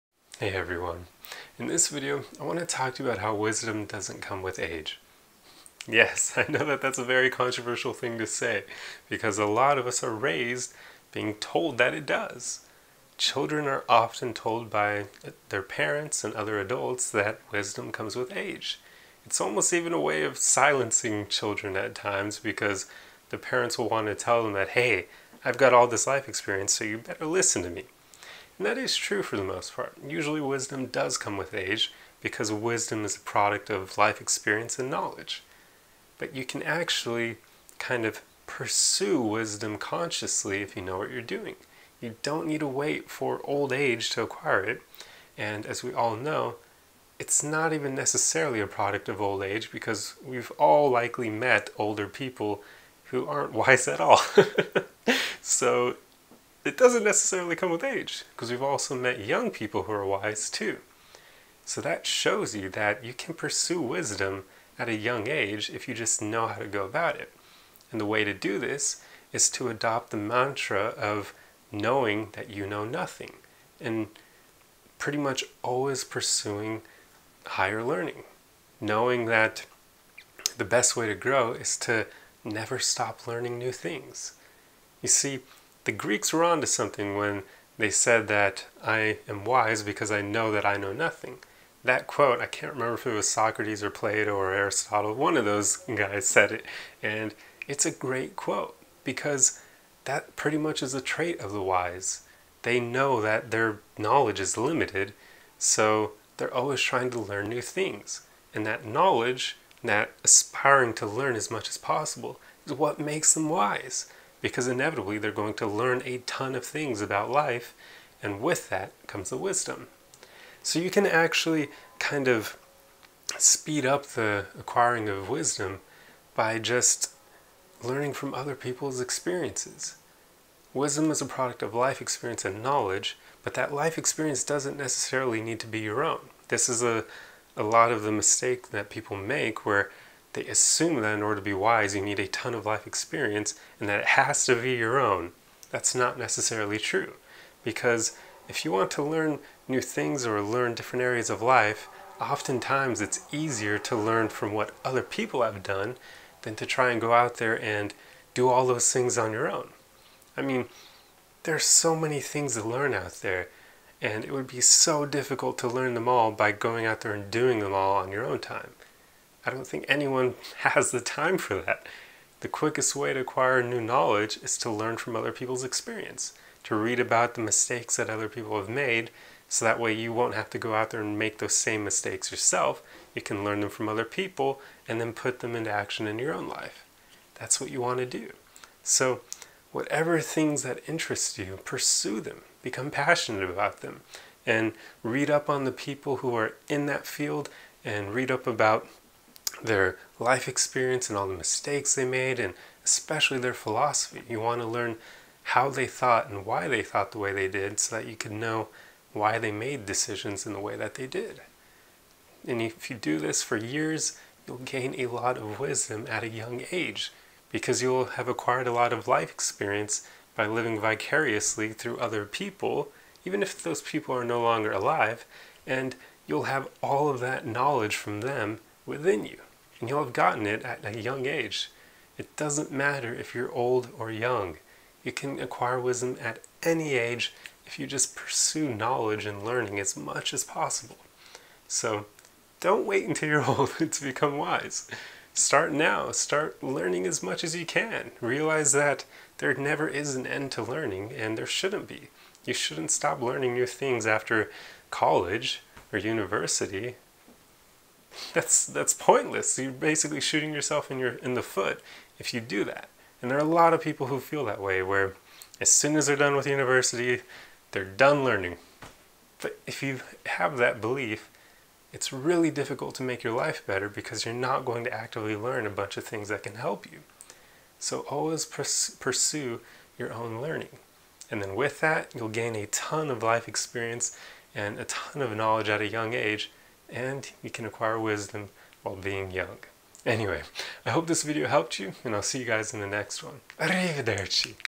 Wisdom Doesn't Come With Age
Video 212/365 of daily motivational and mindset videos. In this video, I talk about how wisdom doesn't necessarily come with age. Therefore, it can be acquired at any age by consciously pursuing it! We've all met old people who weren't wise and young people who were. Wisdom is a product of life experience and knowledge, but both of those things can be acquired at any time. The wise know that their knowledge is limited, so they actively pursue learning new things as much as possible. If you never stop learning, then it will only be a matter of time before you acquire wisdom!